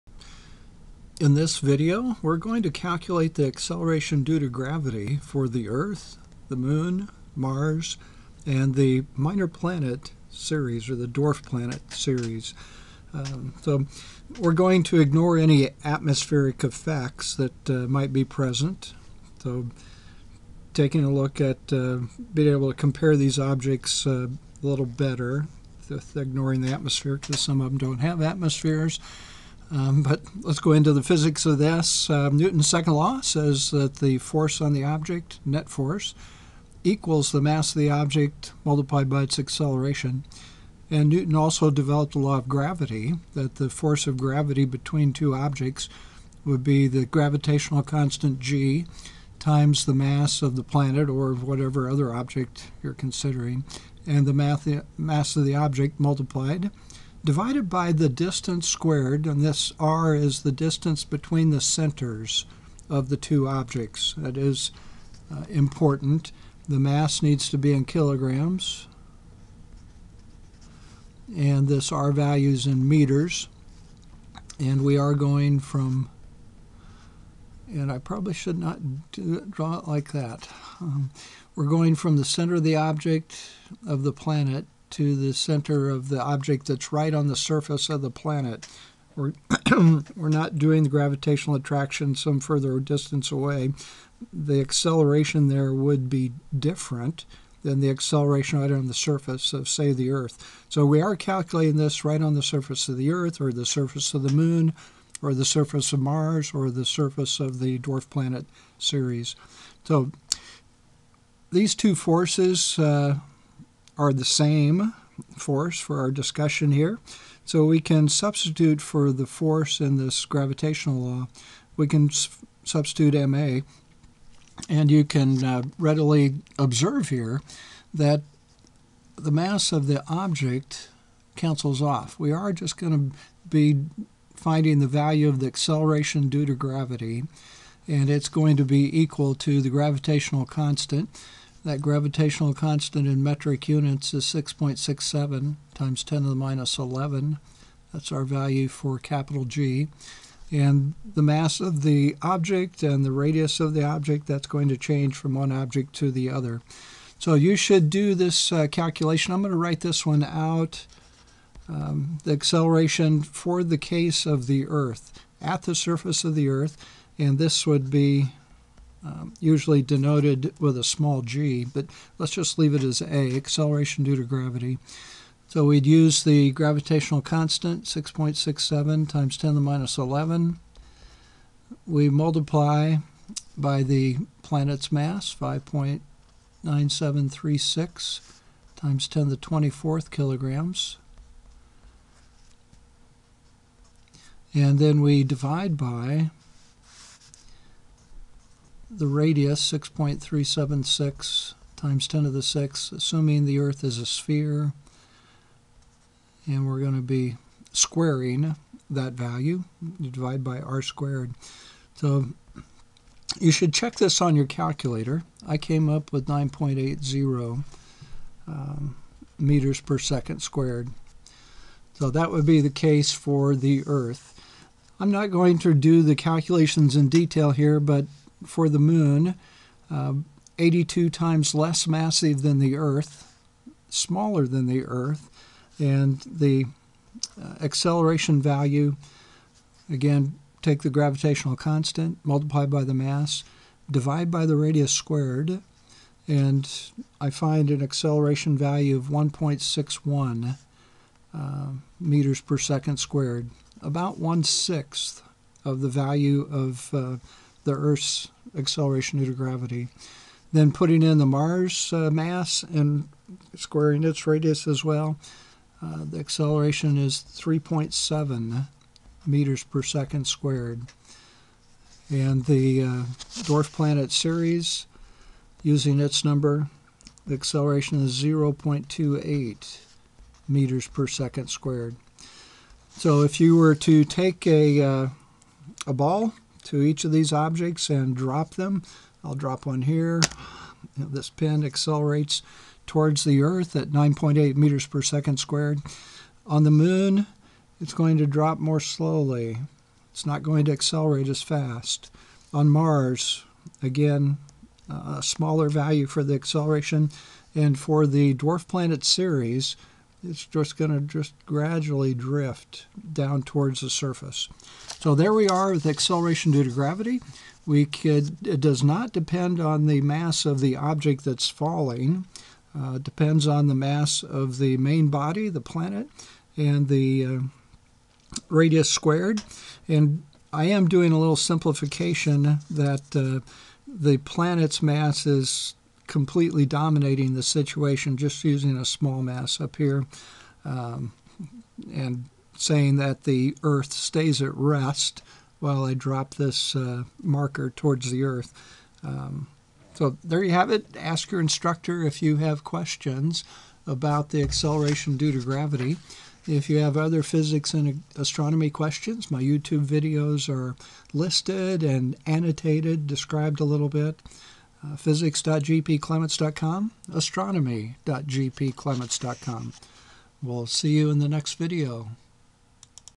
Acceleration Due to Gravity for Earth, Moon, Mars, Ceres
The acceleration due to gravity is calculated for the Earth, Moon, Mars, and Ceres.  Introductory General Astronomy